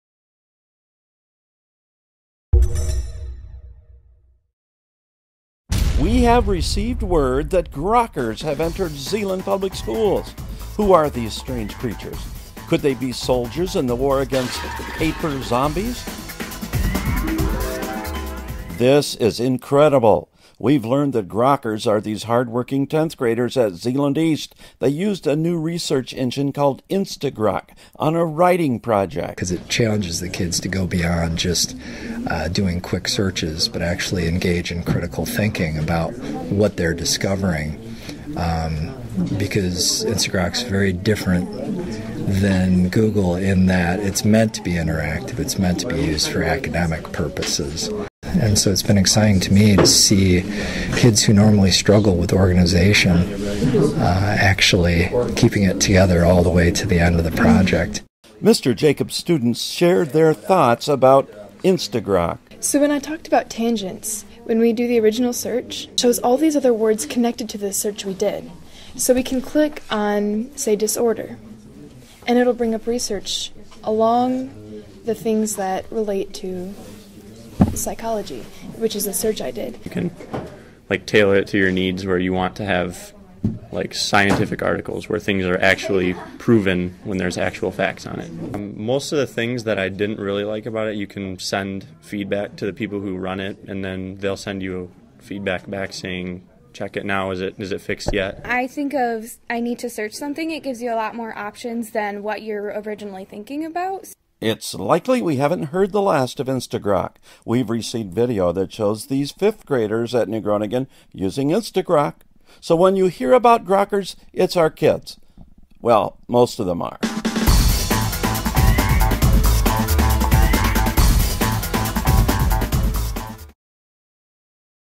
Zeeland "Grokkers" apply critical thinking using a research engine
Zeeland East tenth graders in teacher Shawn Jacobs' class recently used instaGrok, a new search engine, to complete research on a challenging writing assignment. Learn